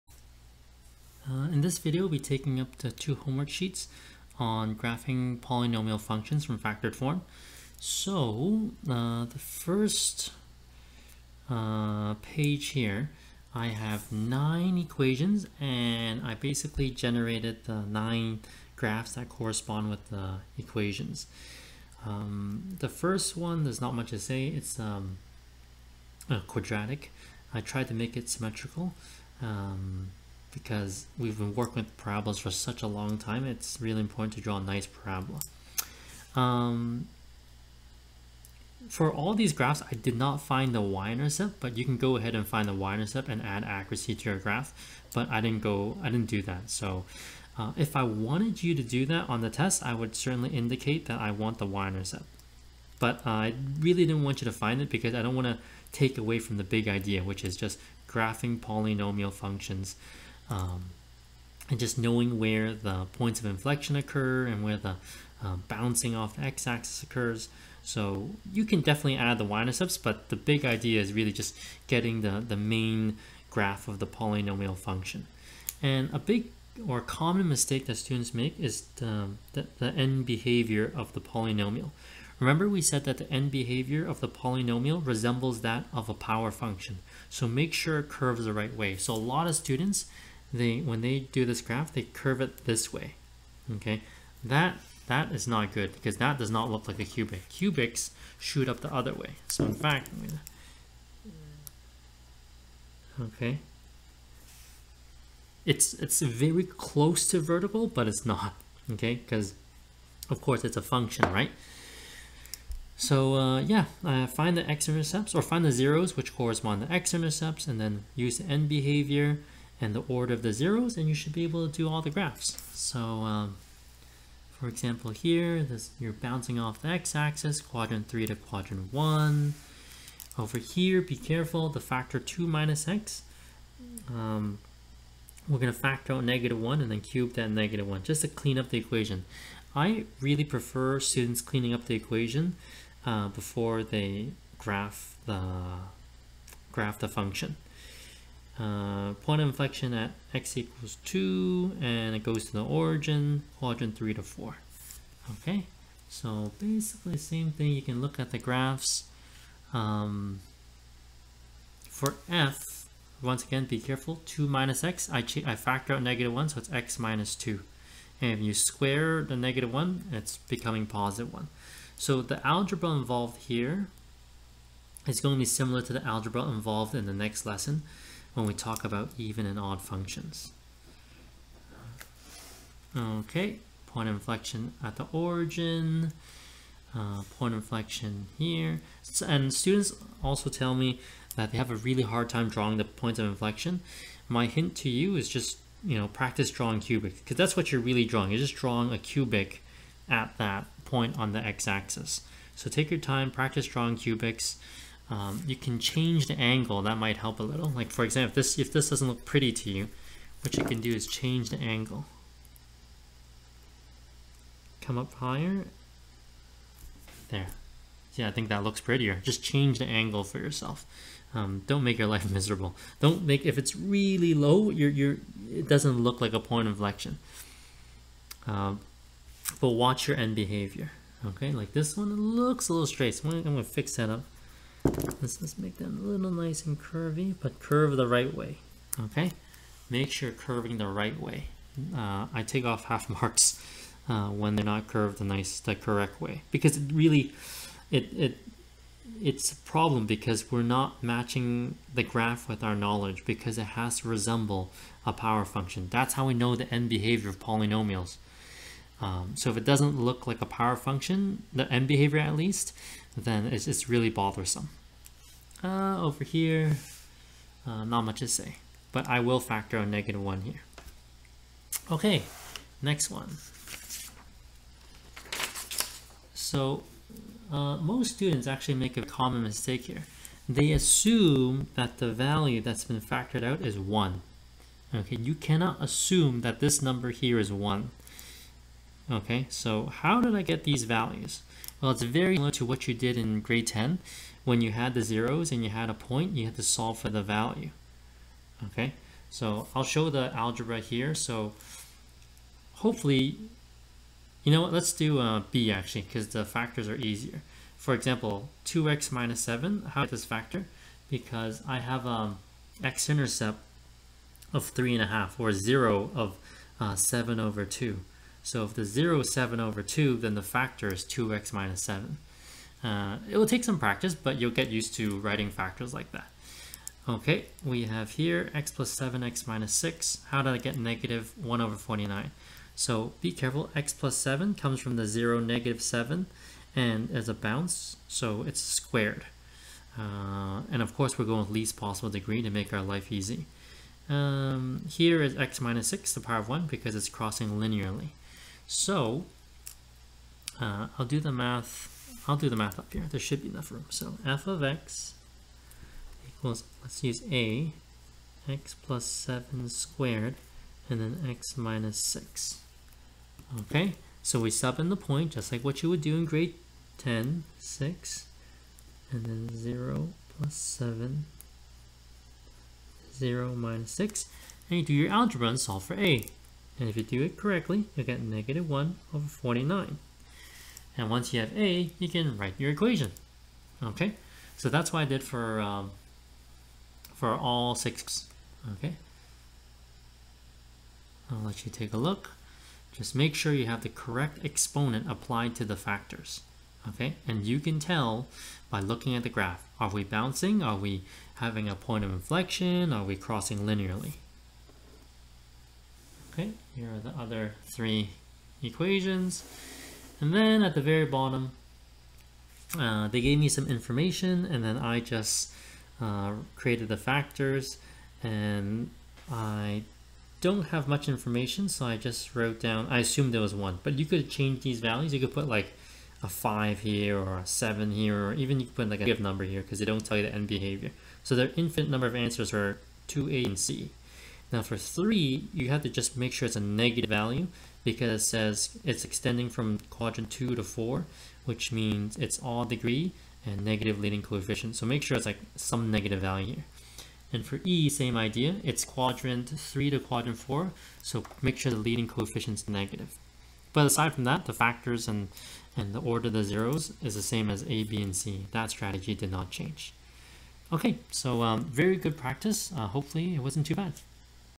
MHF4U 1.5b Equations and Graphs of Polynomial Functions Homework Solutions
Here are the solutions for the Equations and Graphs of Polynomial Functions Homework Sheet.